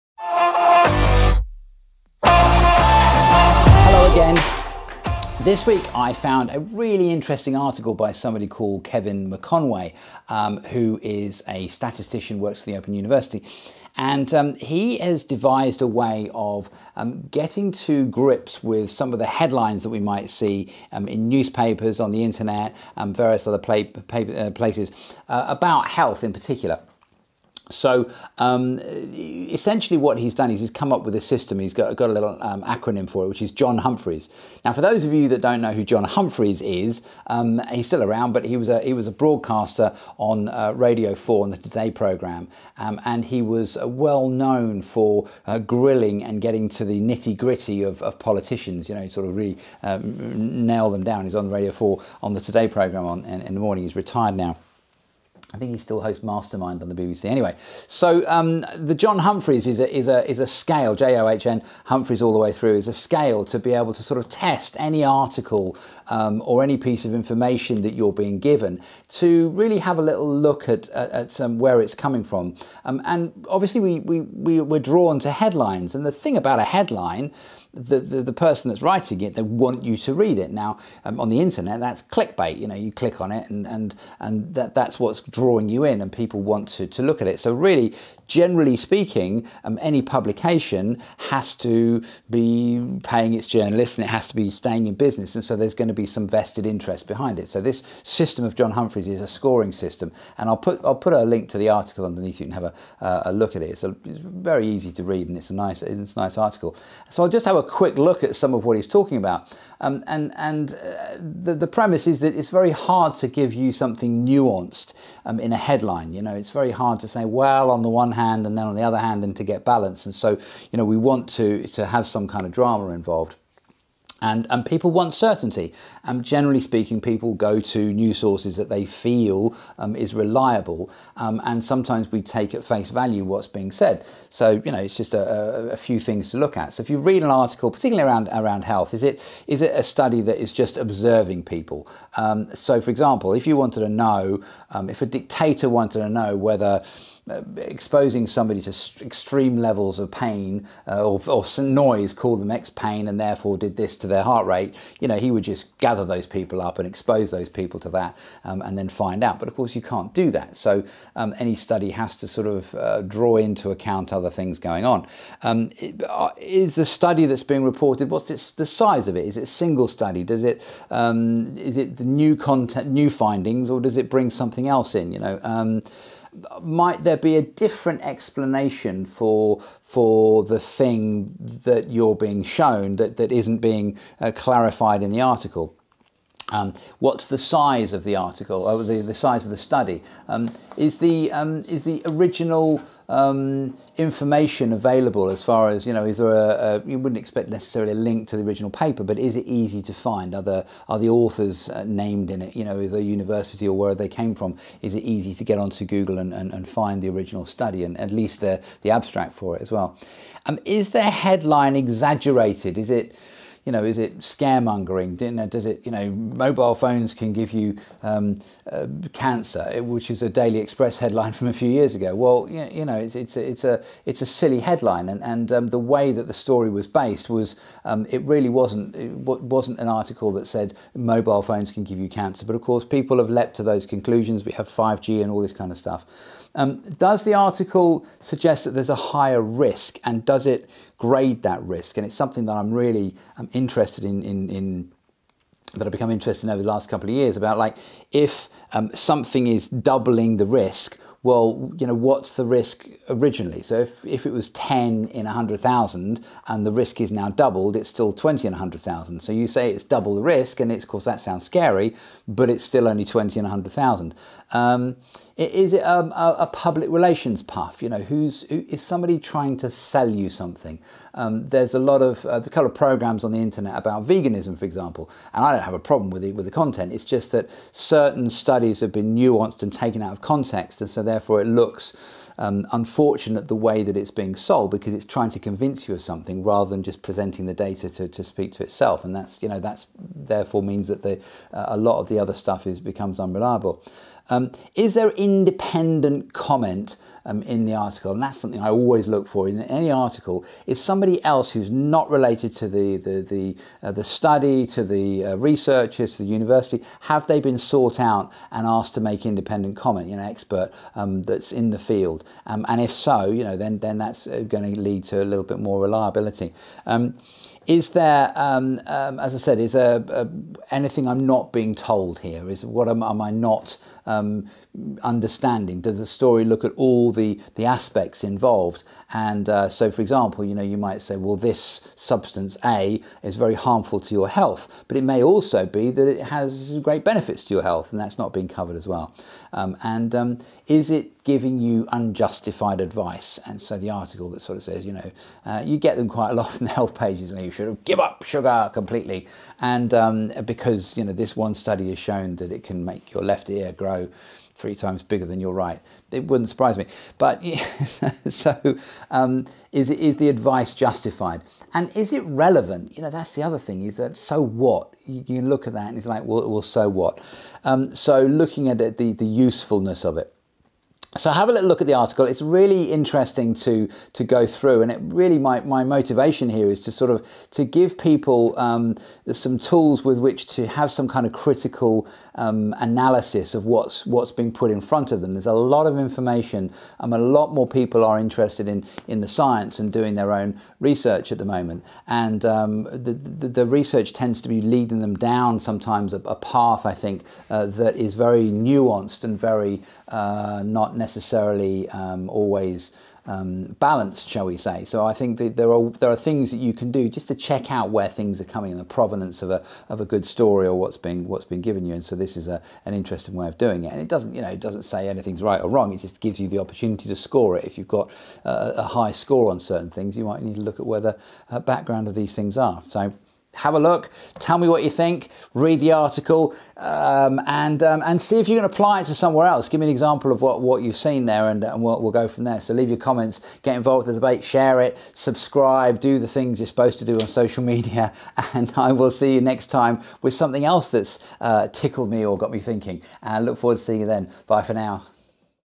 Medical 'Risk' Stories Debunked
It can be hard when sifting through information from newspapers, TV, Internet and so forth,  to work out just whether what we are seeing is reliable.

A really useful and interesting article gives a simple way of scoring information and allowing us to be more discerning about what we read.

Here's the link to the original article.